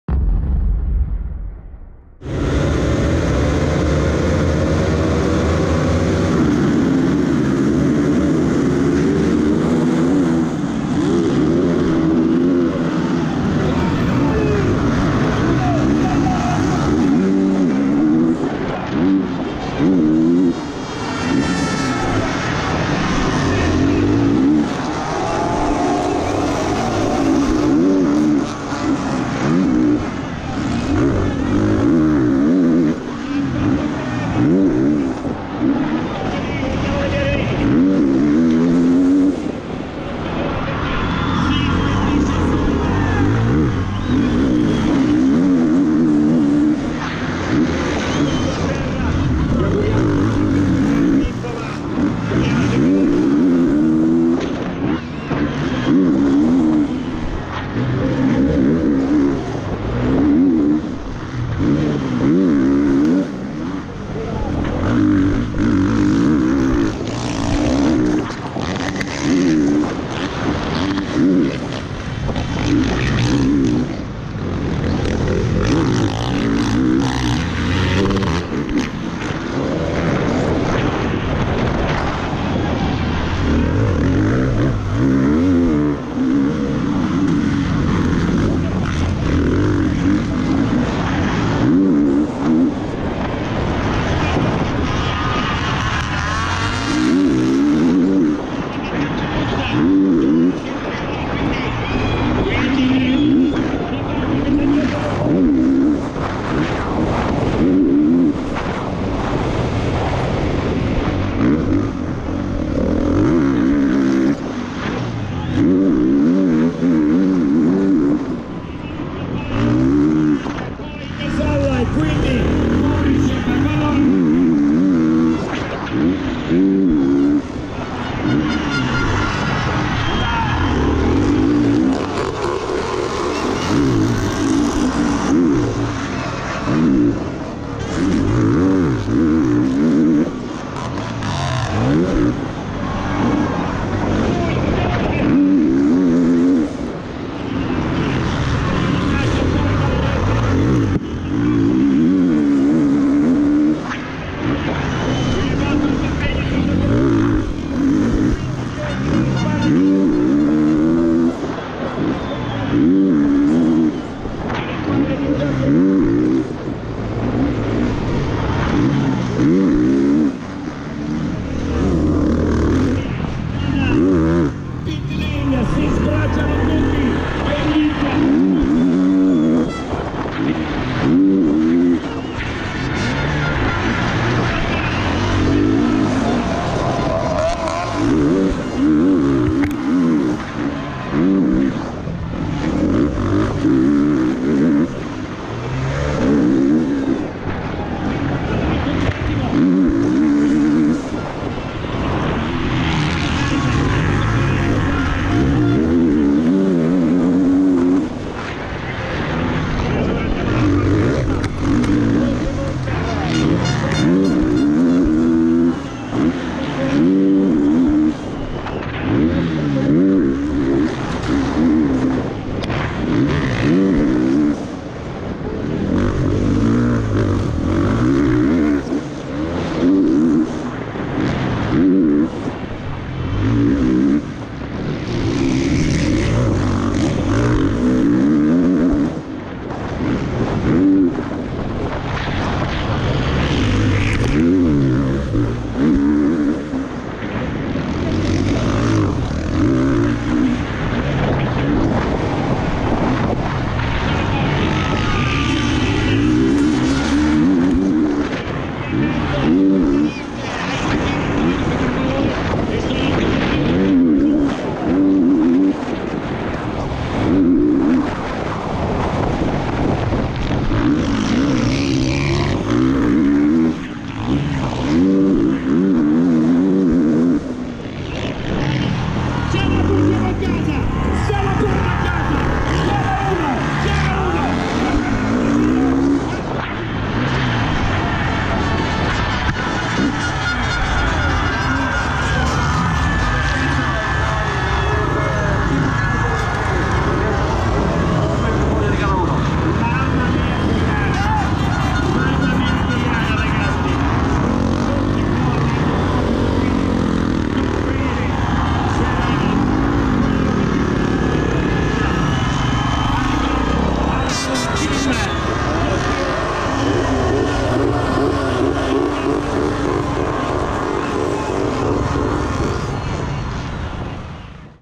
GoPro: Antonio Cairoli FIM MXGP 2017 RD5 Italy Moto 1
Watch Antonio Cairoli first Moto from Round 5 of the 2017 FIM World Motocross championship in Italy!